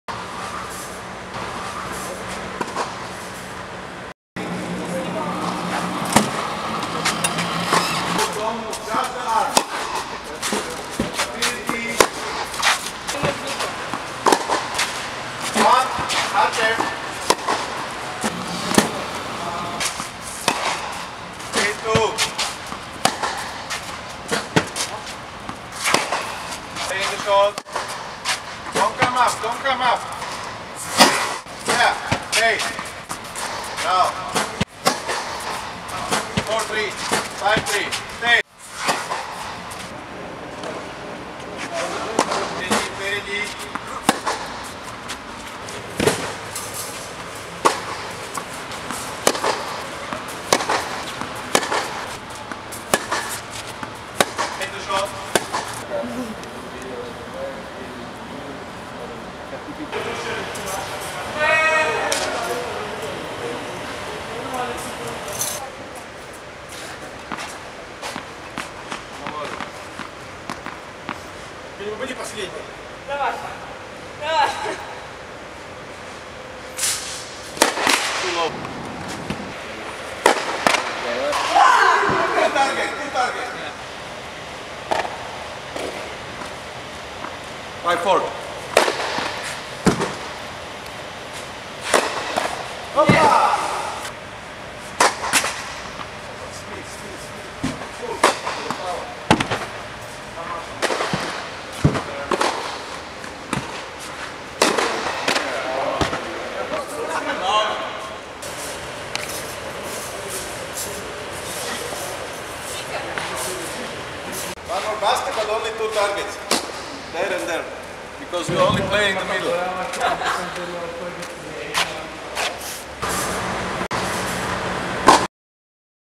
EMPIRE Tennis Academy: Darya Kasatkina and Artem Dubrivnyy in training (video by Brian Dally)
EMPIRE Tennis Academy: Darya Kasatkina and Artem Dubrivnyy in training in Trnava, both are coached by Vladimir Platenik
VIDEO BY BRIAN DALLY